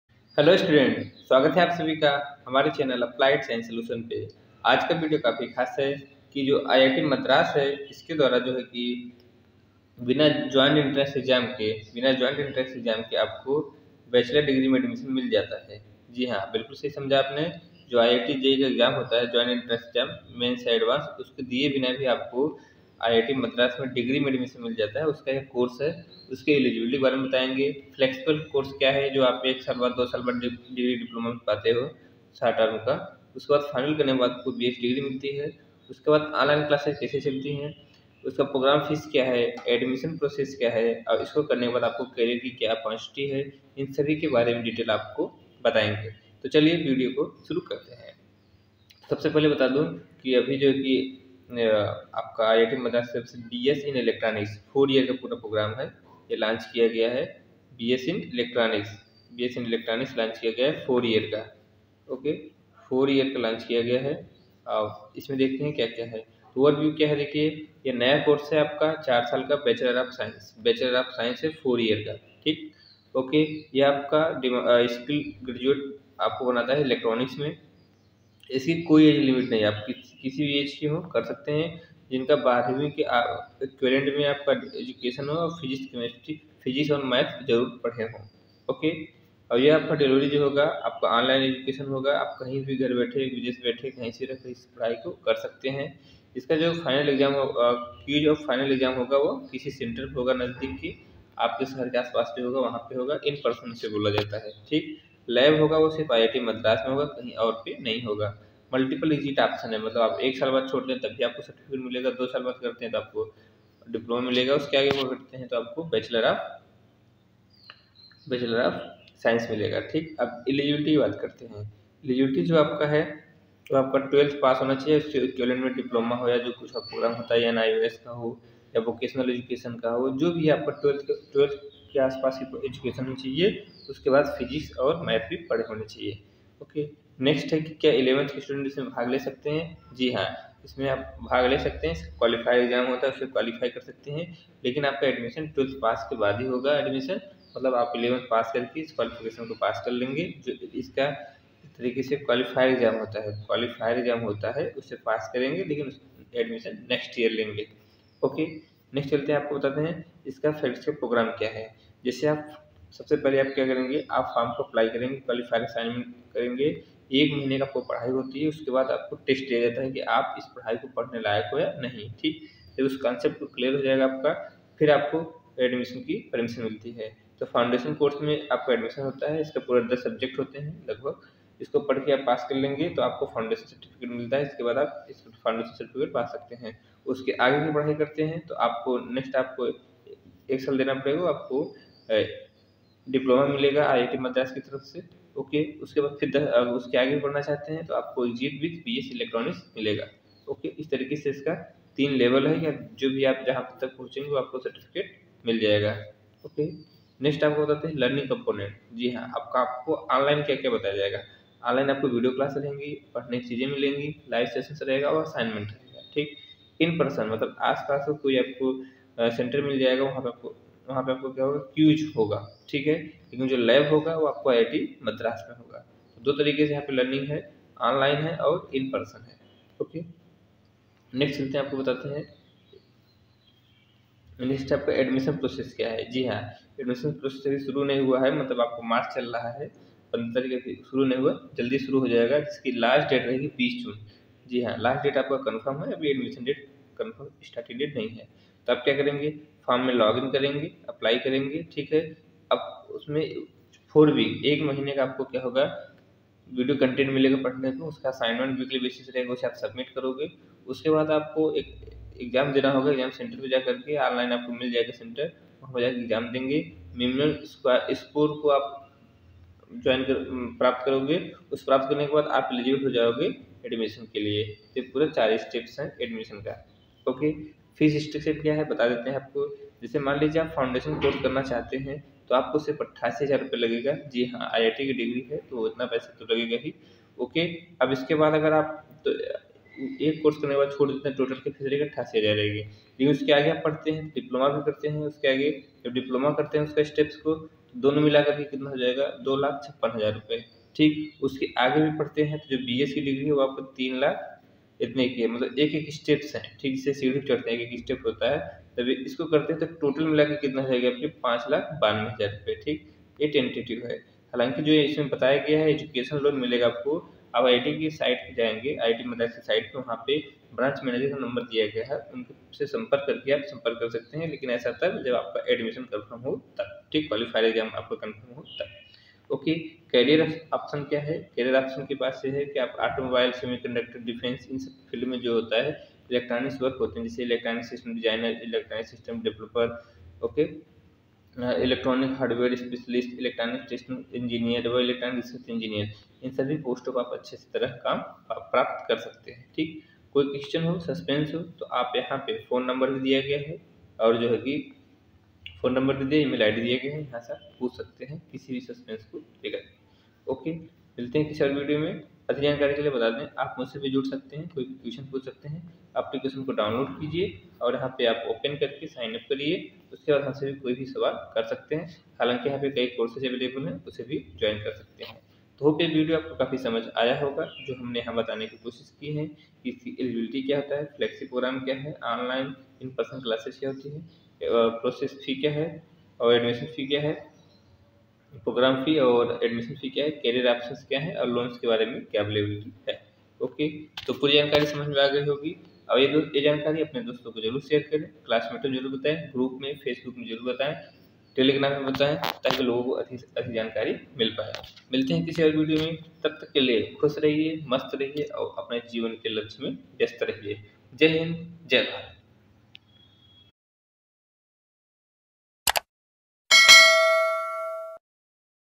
Online 4 Years BS Degree in Electronic Systems by IIT Madras_Video_3
IIT Madras

BS in Electronic Systems

Program Overview

Eligibility?

Flexible Programs
Online/In-person Classes
Program Fees (Tentative)
Admission Process
Career Options

Scholarships/ Educational Loans are possible.

Visit in playlist for All videos Related to BS Degree in Electronic System by IIT MADRAS 👇

IIT Madras Started the BS Degree program in Electronic Systems. This program is designed for students and working professionals from different age groups to give them an opportunity to study from IIT Madras without having to write the JEE. Through this program, we help our learners to get access to a world-class curriculum in Electronic Systems.

IIT Madras, India’s top technical institute, welcomes you to the Four-year Bachelor of Science (BS) Degree in Electronic Systems.
Study from anywhere, at your own pace.

Eligibility to Apply

Anyone who has passed Class 12 or equivalent with Physics and Mathematics can apply irrespective of age.

School students who have appeared for their Class 11 final exams can apply. Those who qualify can join the program after passing Class 12.

View list of accepted class 12 equivalents.👇

1. A Diploma recognised by AICTE or a state board of technical education of at least 3 year duration.

2. Any Public School / Board / University examination in India or in any foreign country recognised as equivalent to the 10+2 system by the Association of Indian Universities (AIU).

3. Final examination of the two-year course of the Joint Services Wing of the National Defence Academy.

4. General Certificate Education (GCE) examination (London / Cambridge / Sri Lanka) at the Advanced (A) level.

5. High School Certificate Examination of the Cambridge University or International Baccalaureate Diploma of the International Baccalaureate Office, Geneva.

6. Higher Secondary Certificate vocational examination.600036

7. Intermediate or two-year Pre-University examination conducted by a recognised Board / University.

8. Senior Secondary School Examination conducted by the National Institute of Open Schooling with a minimum of five subjects.

DEGREE PROGRAM

B.S. in Electronic Systems
In this program, students will have the unique opportunity to develop industry-ready skills to design and build Electronic and embedded Systems.

DIPLOMA PROGRAM

Diploma in Electronic Systems
The Diploma is intended for anyone looking to grow their careers in the electronic systems industry or who wants to learn more in the field of electronic systems.
Apply for and join the Degree Program (BS in Electronic Systems) and exit after completing the Diploma Level courses with a Diploma in Electronic Systems

IITM BS Degree Office, 3rd Floor, ICSR Building,
IIT Madras, Chennai - 600036

Overall Structure

The program comprises three levels and a learner has to successfully complete all three levels to get the BS Degree from IIT Madras.

There is flexibility to exit at any level. Depending on the courses completed and credits earned, the learner can receive a Foundation Certificate from IITM CODE (Centre for Outreach and Digital Education) or Diploma from IIT Madras or BS Degree in Electronic Systems from IIT Madras.

Frequently Asked Questions related to Four-year Bachelor of Science (BS) Degree in Electronic Systems.👇